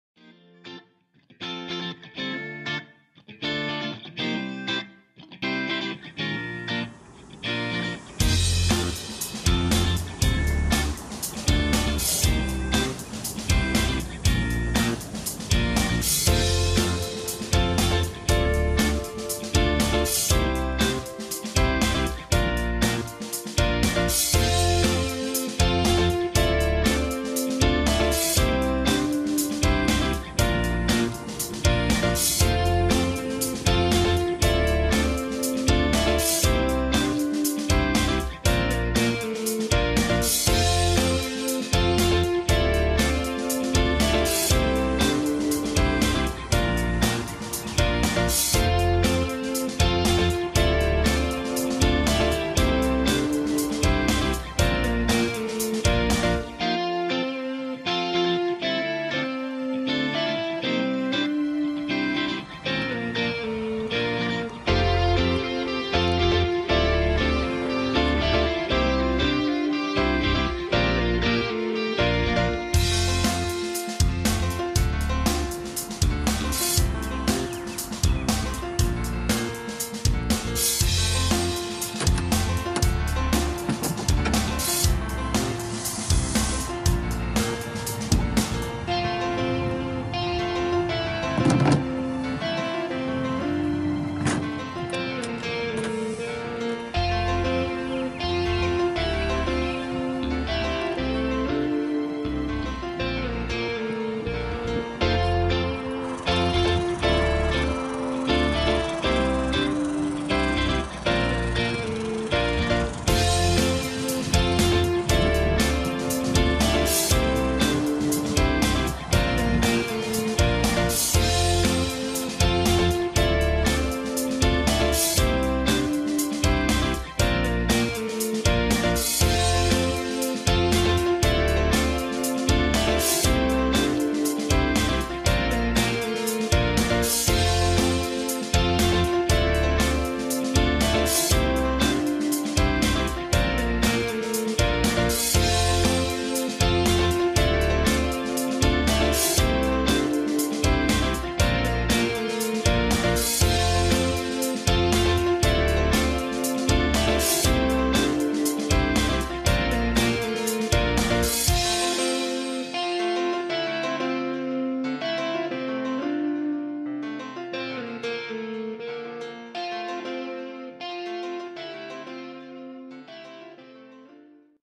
ARGO Conquest 8X8 Lineman XTI - Overview by Shank's Argo
The ARGO Conquest 8X8 Lineman XTI is the ultimate all-terrain vehicle designed for demanding utility tasks.

SHANK'S ARGO is one of the largest stocking Argo XTV dealers in the Mid-Atlantic region.